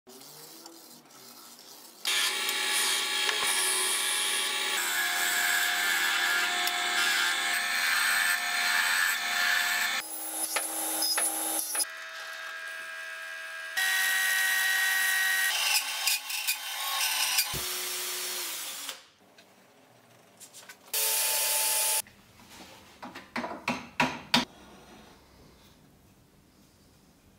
In this video I make a DIY compass
In this video I make a DIY compass. Σε αυτό το video φτιάχνω έναν DIY δσαβήτη.

Παραθέτω ενδεικτικά μερικά από τα εργαλεία (ή παρόμοια) που χρησιμοποιησα.
Tools and materials I propose:
Wood turning tools set
Chuck kit
Tormek T8 Sharpening System
Tormek Woodturner’s Kit
Flat file
Caliper set
Table saw
Chisel set
Forstner bit set
Drill press